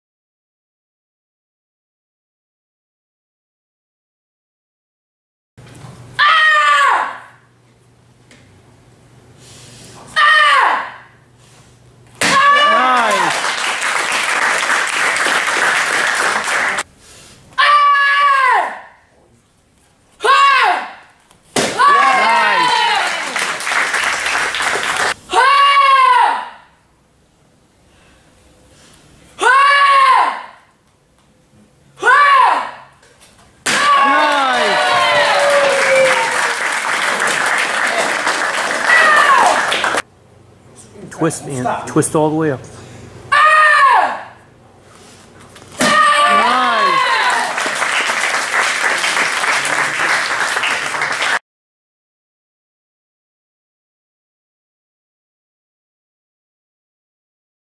Anthony Breaking
Anthony Le breaking for his Black Belt Test on March 1st 2019